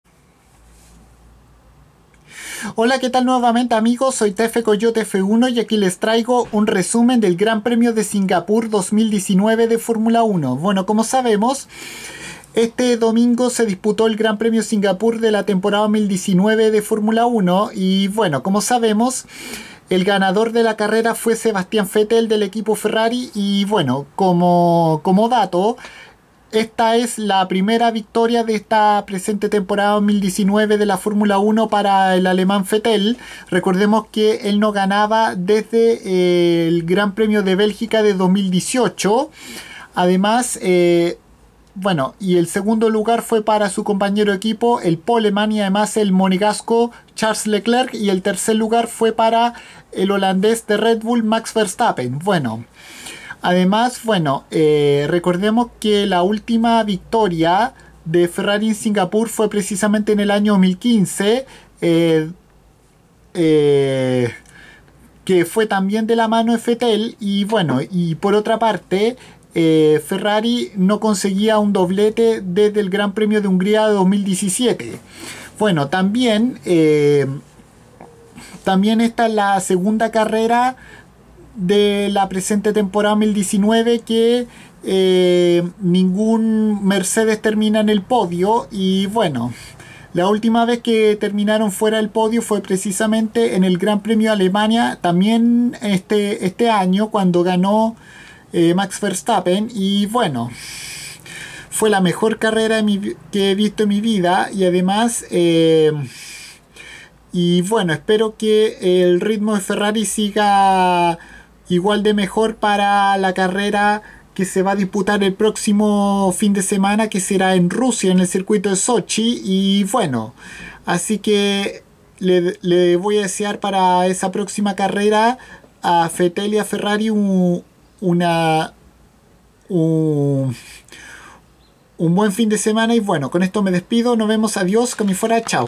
Resumen GP de Singapur 2019 de F1
Un breve resumen de lo que fue el GP nocturno de Singapur de la Temporada 2019 de la Fórmula 1 en la que Sebastian Vettel de Ferrari obtuvo su primera victoria del año(que no ganaba Bélgica 2018) seguido de su compañero el poleman monegásco Charles Leclerc 2° y el holandés de Red Bull Racing Honda Max Verstappen

Ademas es el primer doblete de la Scuderia desde Hungría 2017 y también la 3° victoria de Ferrari en Singapur(la última fue en 2015 también con Vettel)

También es la segunda ocasión en este año que un Mercedes no sube al podio(la última fue en Alemania)